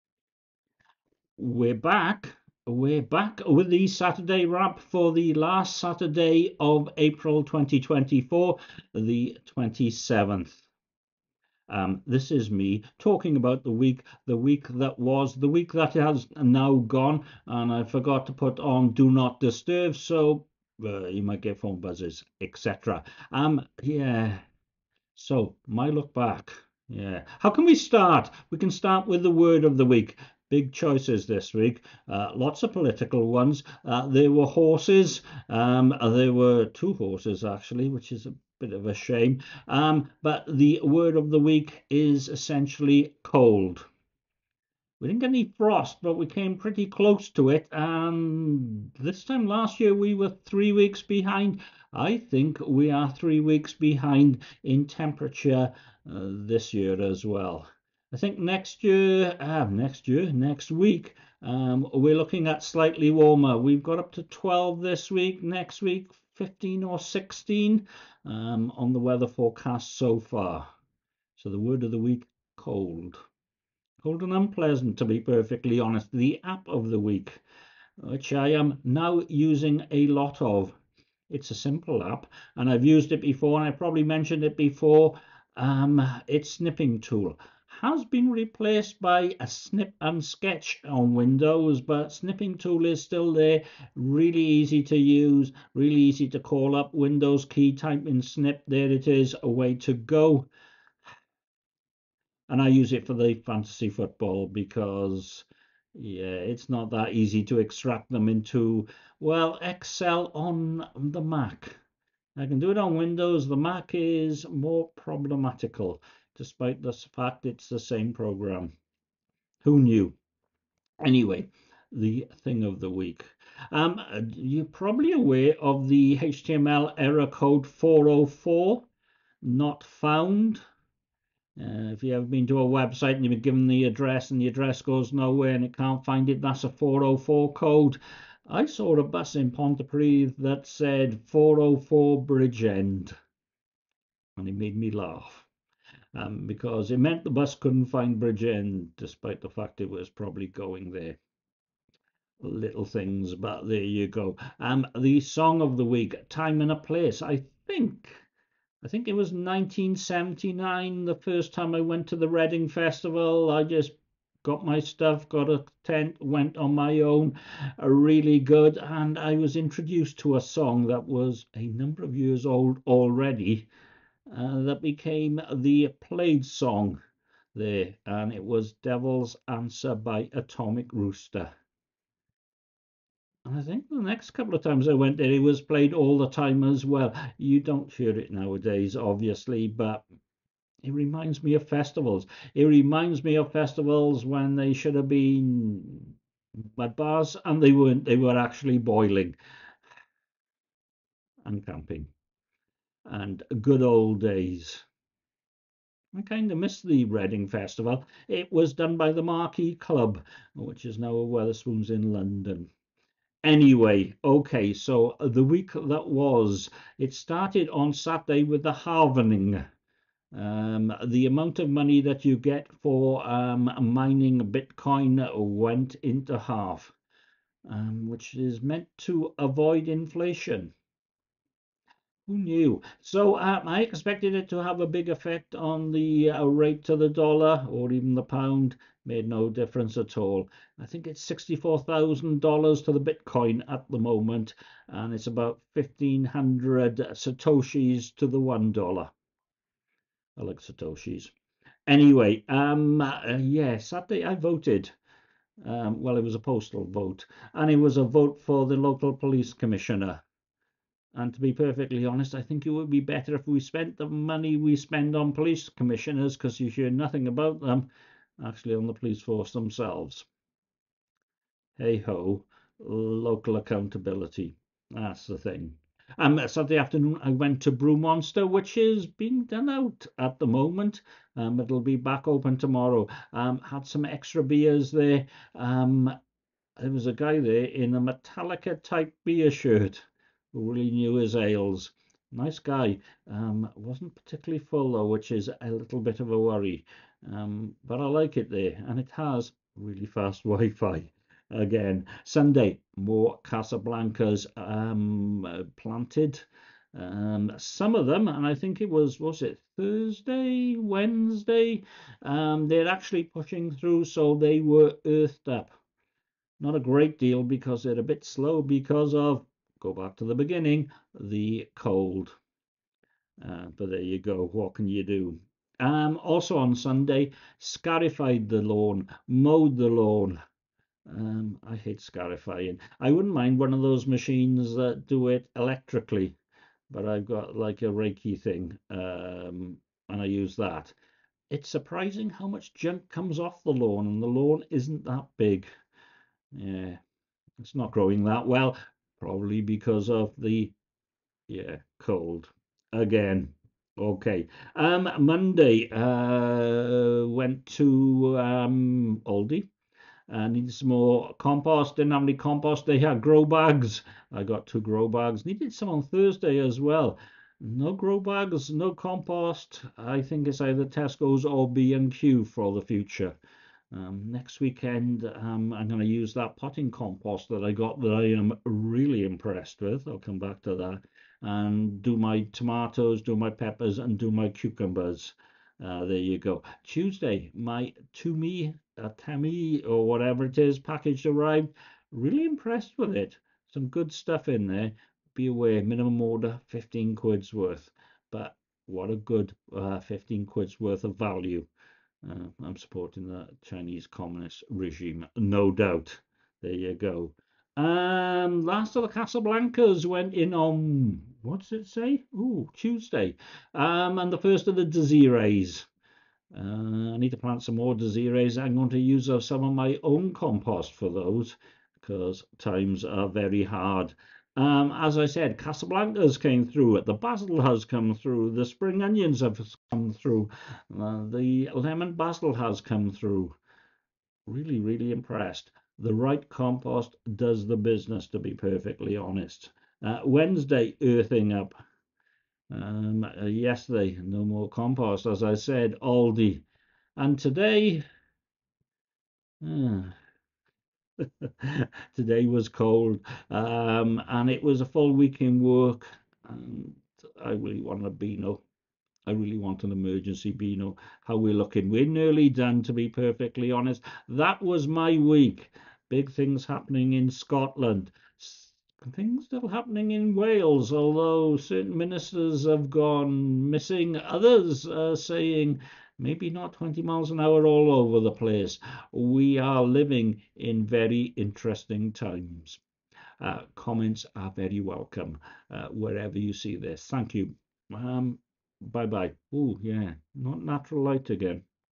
For Saturday 27 April 2024
The Saturday Wrap for 27 April 2024. Cold, Snipping Tool, 404, Atomic Rooster, Halving, Voted, Brew Monster, Planting, Mow and Scarify, Growbags, Temu, Desiree, pushing through, Earthing up, Aldi.